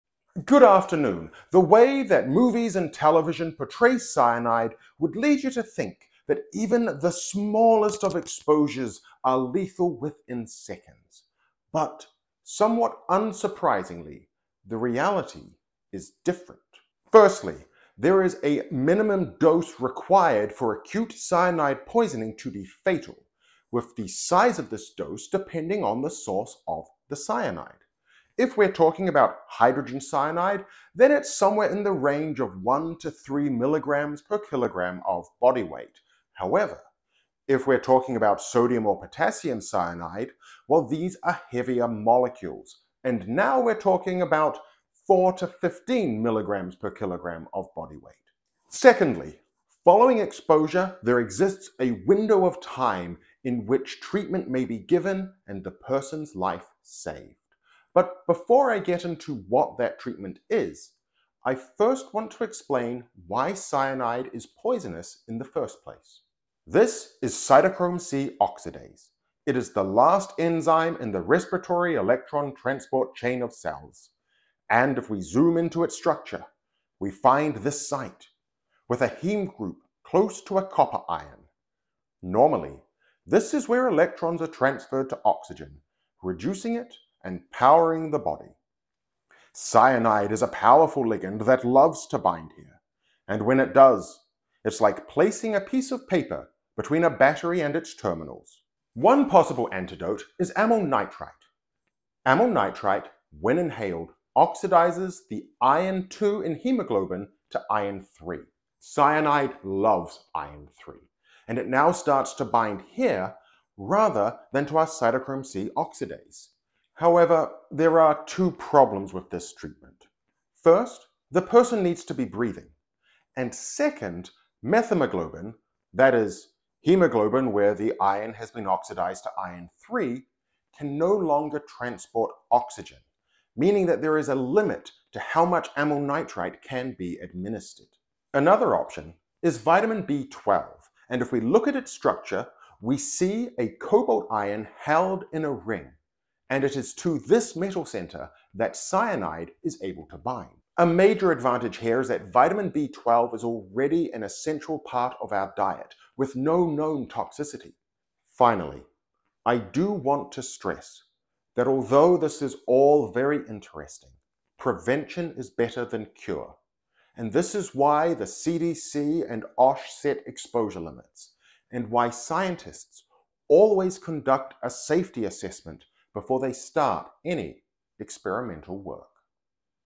How Cyanide Poisoning Is Treated
Cyanide is a powerful cytochrome c oxidase inhibitor. It shuts down the electron transport process in cells with potentially fatal results. Luckily, it is possible to treat cyanide poisoning by providing alternate metal centers for it to bind to.